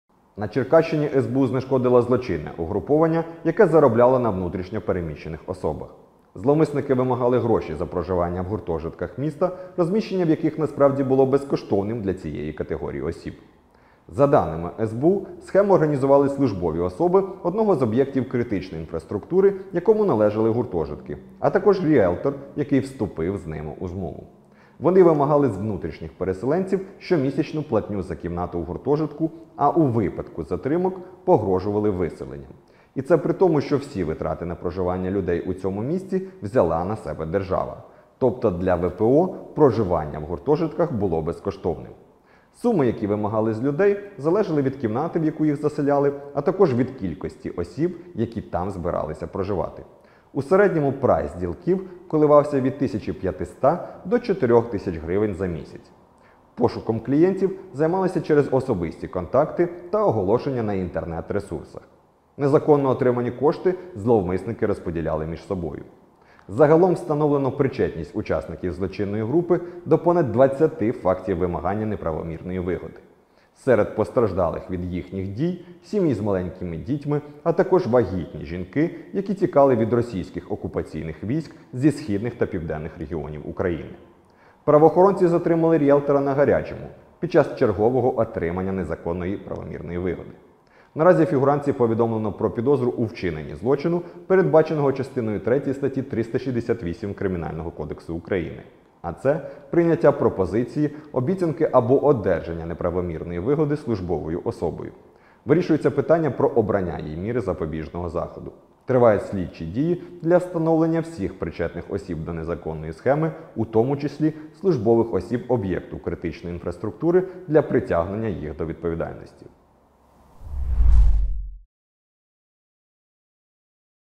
СБУ викрила ділків, які намагалися «нажитися» на переселенцях вимагали оплату за безкоштовне житло
На Черкащині СБУ знешкодила злочинне угруповання, яке «заробляло» на внутрішньо переміщених особах. Зловмисники вимагали гроші за проживання в гуртожитках міста, розміщення в яких насправді було безкоштовним для цієї категорії осіб.

За даними СБУ, схему організували службові особи одного з об’єктів критичної інфраструктури, якому належали гуртожитки, а також рієлтор, який вступив з ними у змову.

Вони вимагали з внутрішніх переселенців щомісячну платню за кімнату в гуртожитку, а у випадку затримок – погрожували виселенням. І це при тому, що всі витрати на проживання людей у цьому місці взяла на себе держава. Тобто для ВПО проживання в гуртожитках було безкоштовним.

Суми, які вимагали з людей, залежали від кімнати, в яку їх заселяли, а також від кількості осіб, які там збиралися проживати. У середньому прайс ділків коливався від 1,5 до 4 тис. грн за місяць.

Пошуком «клієнтів» займалися через особисті контакти та оголошення на інтернет-ресурсах. Незаконно отримані кошти зловмисники розподіляли між собою.

Загалом встановлено причетність учасників злочинної групи до понад 20 фактів вимагання неправомірної вигоди. Серед постраждалих від їхніх дій – сім’ї з маленькими дітьми, а також вагітні жінки, які тікали від російських окупаційних військ зі східних та південних регіонів України.

Правоохоронці затримали рієлтора «на гарячому» під час чергового отримання неправомірної вигоди.

Наразі фігурантці повідомлено про підозру у вчиненні злочину, передбаченого ч. 3 ст. 368 Кримінального кодексу України (прийняття пропозиції, обіцянки або одержання неправомірної вигоди службовою особою). Вирішується питання про обрання їй міри запобіжного заходу.

Тривають слідчі дії для встановлення всіх причетних осіб до незаконної схеми, у тому числі службових осіб об’єкту критичної інфраструктури для притягнення до відповідальності.

Заходи з припинення протиправної діяльності проводились спільно зі слідчими Національної поліції за процесуального керівництва Черкаської окружної прокуратури та сприяння Черкаської міської ради.